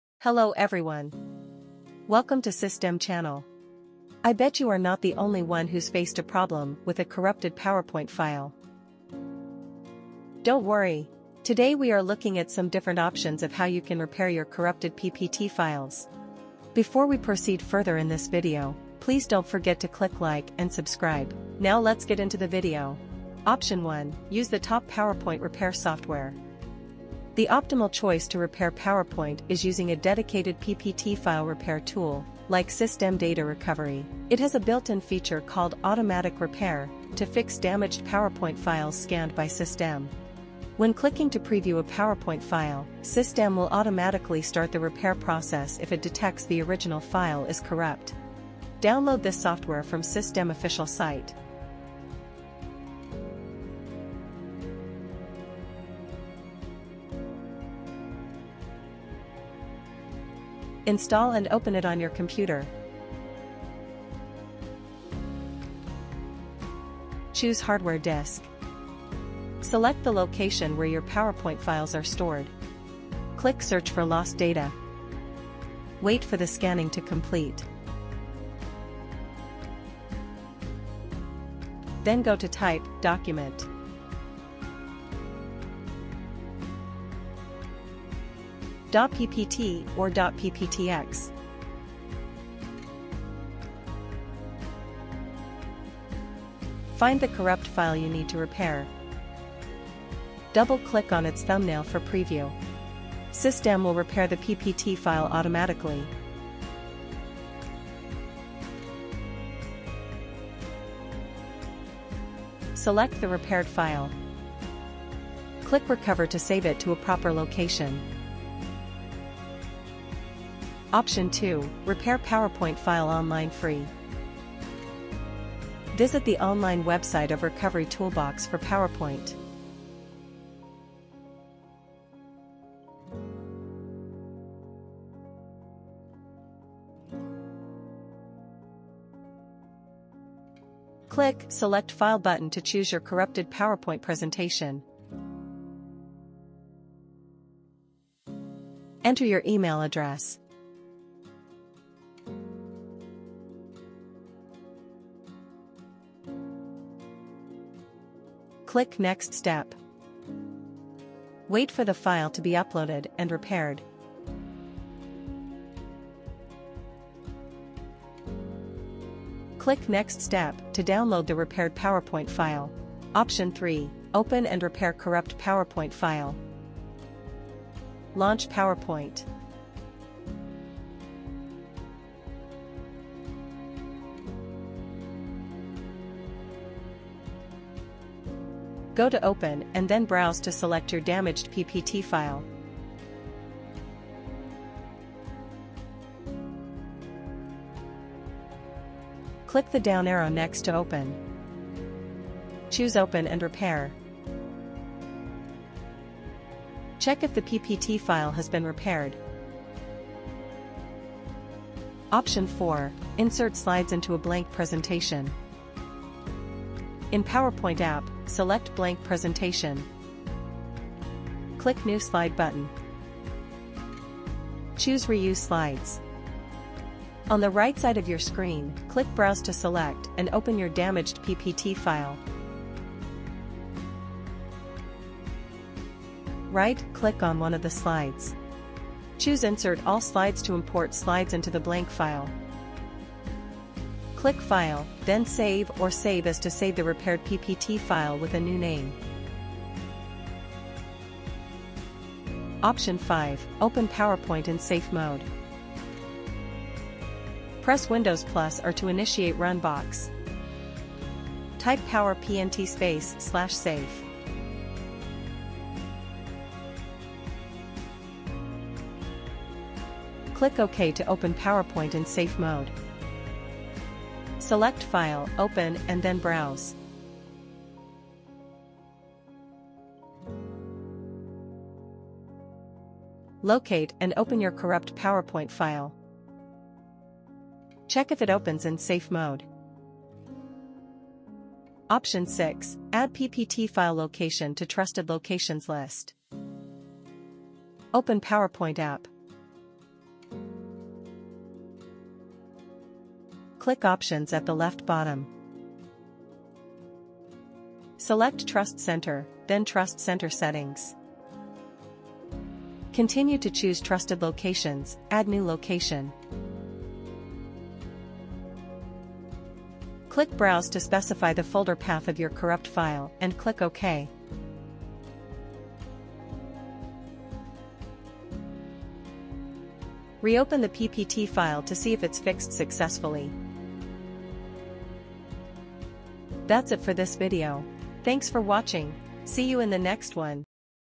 6 Best PowerPoint Repair Options for Windows and Mac (Online Free)
This video explores 6 ways to repair PowerPoint files on Windows or Mac, including online and free options.
⌚Timestamps:
00:25 Option 1: Use the top PowerPoint repair software
02:02 Option 2: Repair PowerPoint file online free
02:54 Option 3: Open and Repair corrupt PowerPoint file
03:34 Option 4: Insert slides into a blank presentation
04:15 Option 5: Open PowerPoint in Safe Mode
04:54 Option 6: Add PPT file location to trusted locations list